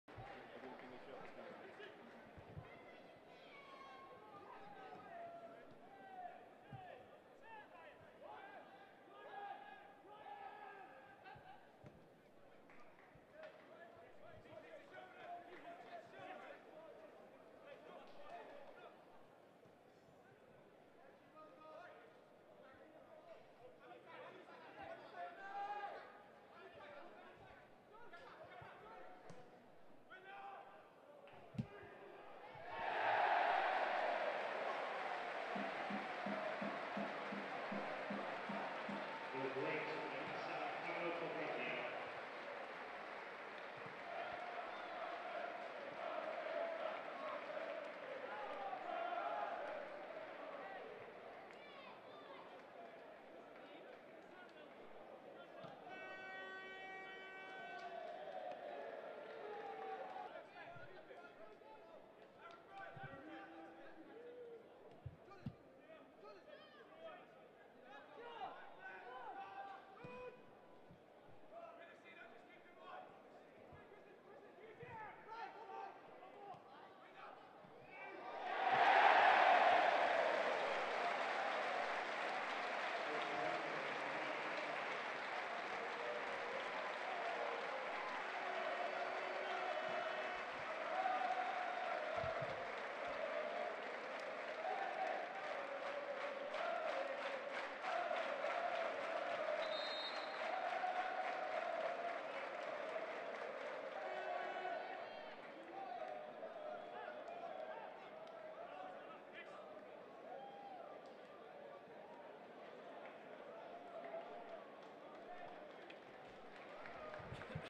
Wycombe Wanderers 0-2 Reading (Pogrebnyak, Taylor)
Goals from the Royals pre season win over Wycombe Wanderers. Pavel Pogrebnyak scored the first after great work from Jordan Obita, whilst Ryan Edwards set up Jake Taylor for a deft finish to make it 2-0.